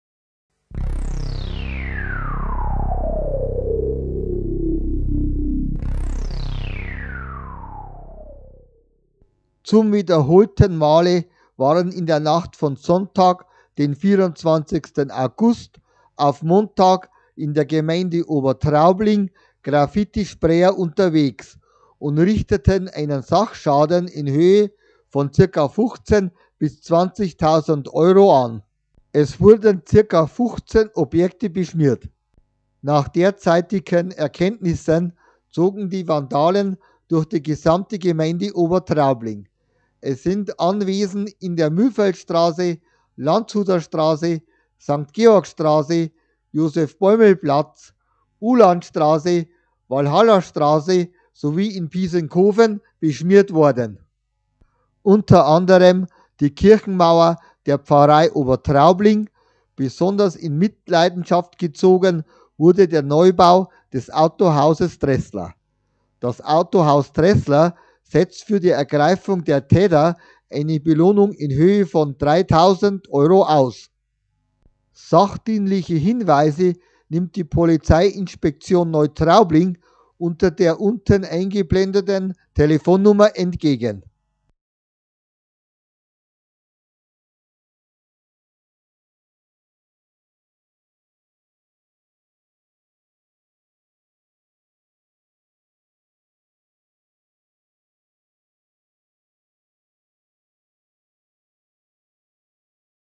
Graffiti Sprayer in Obertraubling unterwegs:
Zum wiederholten male waren in der Nacht von Sonntag, den 24. August auf Montag in der Gemeinde Obertraubling Graffiti-Sprayer unterwegs und richteten einen Sachschaden in Höhe
von ca. 15.000,-- und 20.000,-- Euro an.
Es wurden ca 15 Objekte an verschiedenen Örtlichkeiten beschmiert.
Unter anderem die Kirchenmauer der Pfarrei Obertraubling. Besonders in Mitleidenschaft gezogen wurde der Neubau des Autohauses Dressler.
Das Autohaus Dressler setzt für die Ergreifung der  Täter eine Belohnung in Höhe von
3.000,-- Euro aus.
Sachdienliche Hinweise nimmt die PI Neutraubling, Tel. 09401/93020, entgegen.